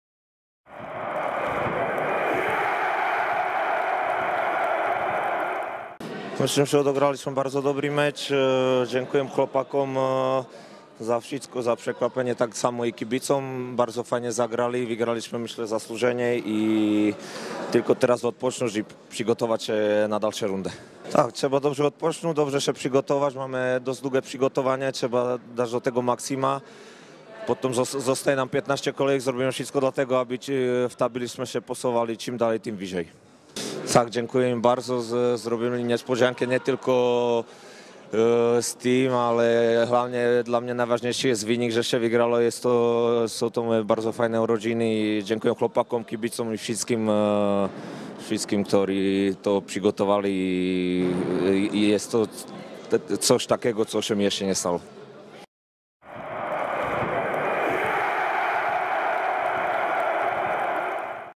Śląsk Wroclaw | Marian Kelemen - wypowiedź po meczu z Legią Warszawa
Wypowiedź bramkarza Śląska Wrocław Mariana Kelemena po wygranym 1:0 spotkaniu z Legią Warszawa w 15. kolejce T- Mobile Ekstraklasy (7 grudnia 2012 roku).